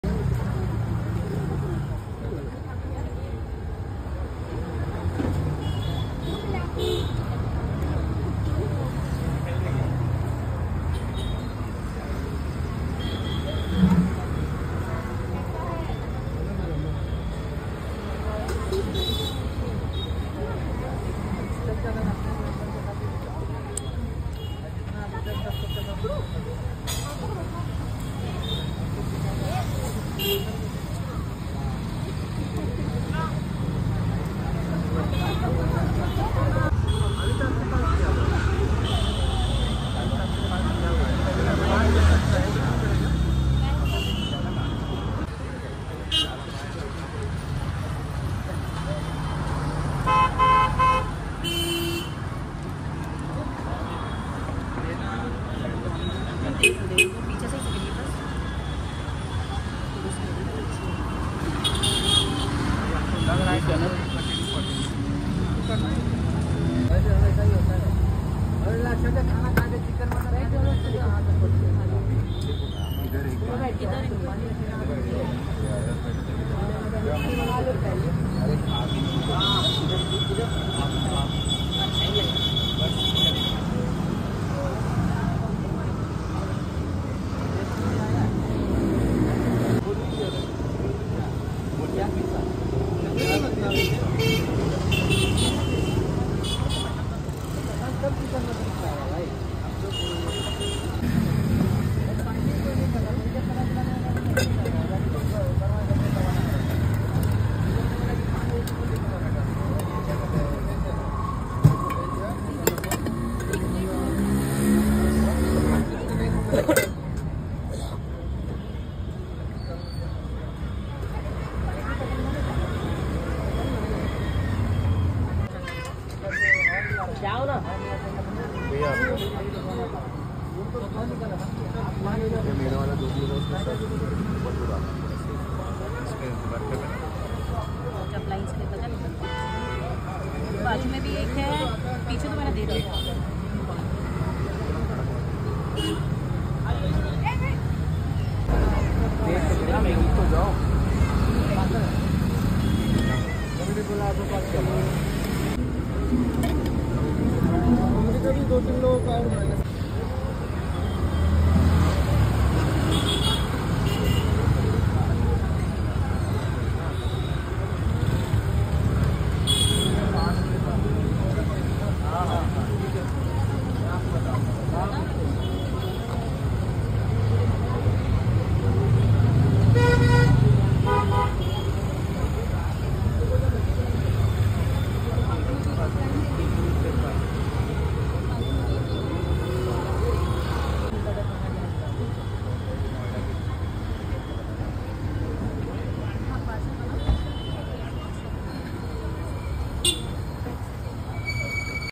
Bihari Style Litti Chokha in Nagpur Just For 50/- Rs Only | Futala Street Food
Price : 50/- Rs Plate

Timing : 5.30PM To 8PM (Only Saturday & Sunday)

📍Location: Vayusena Nagar Road, Futala Lake, Nagpur